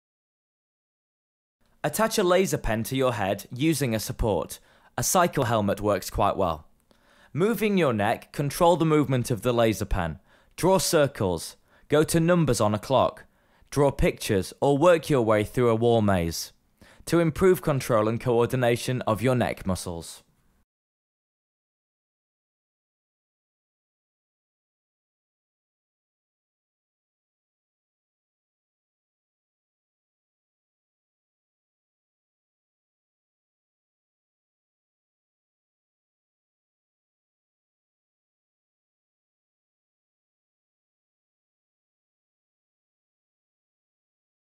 Cervical Proprioception LASER Pen Game
Attach a LASER-pen to your head using a support (a cycle helmet works quite well). Moving your neck, control the movement of the LASER pen. Draw circles, go to numbers on a clock, draw pictures or work your way through a wall maze, to improve control and co-ordination of your neck muscles.